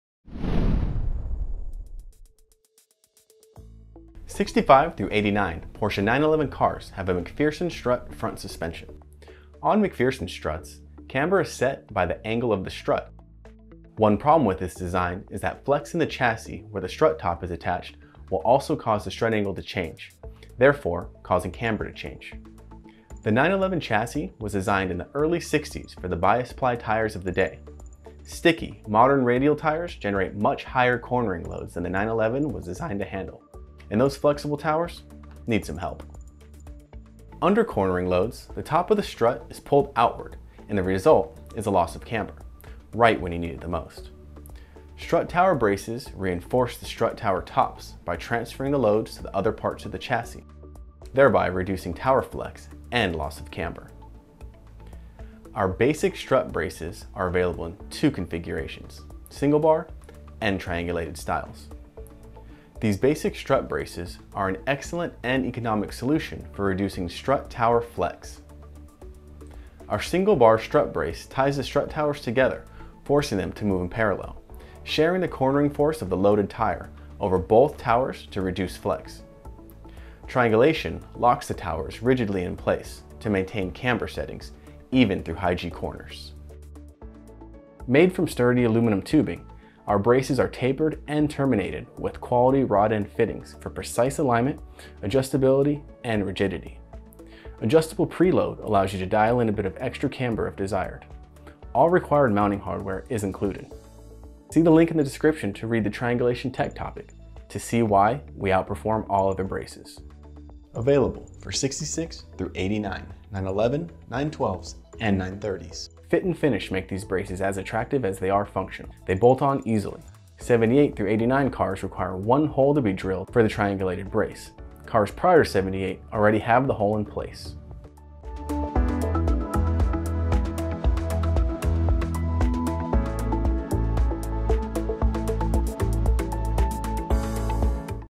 Basic Strut Brace Bar for Porsche 91, 912, & 930 Elephant Racing Suspension Products
Elephant Racing Basic Strut Bar designed for 65-89 Porsche 911 cars that have a Macpherson strut front suspension. On Macpherson struts, the camber is set by the angle of the strut. One problem with this design is that flex in the chassis where the strut top is attached will also cause the strut angle to change and therefore camber to change.

The 911 chassis was designed in the early 60s for the bias-ply tires of the day. Sticky modern radial tires generate much higher cornering loads than the 911 was designed to handle and those flexible towers need some help.
Under cornering loads, the top of the strut is pulled outward and the result is a loss of camber - right when you need it most. Strut tower braces reinforce the strut tower tops by transferring the loads to other parts of the chassis, thereby reducing tower flex and loss of camber

Our basic strut braces are available in 2 configurations: single bar and triangulated styles. These basic strut braces are an excellent and economic solution for reducing strut tower flex.

Our single bar strut brace ties the strut towers together forcing them to move in parallel, sharing the cornering force of the loaded tire over both towers to reduce flex. Triangulation locks the towers rigidly in place to maintain camber settings – even through high G corners.

Made from sturdy aluminum tubing, our braces are tapered and terminated with quality rod-end fittings for precise alignment, adjustability, and rigidity. Adjustable pre-load allows you to dial in a bit of extra camber if desired. All required mounting hardware is included.

Available for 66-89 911, 912, and 930’s Fit and finish make these braces as attractive as they are functional. They bolt on easily. ’78 to ’89 cars require one hole to be drilled for the triangulated brace. Cars prior to ’78 already have the hole in place.